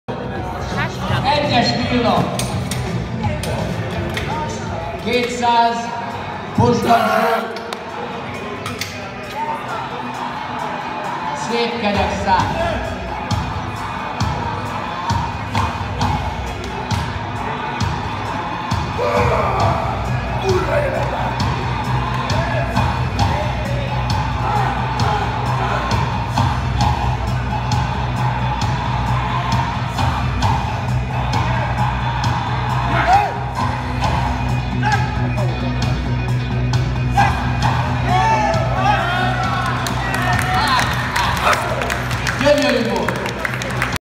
Bench press raw 200kg
Gruber's raw fekvenyomó bajnokság, open. 2. fogás. 200kg.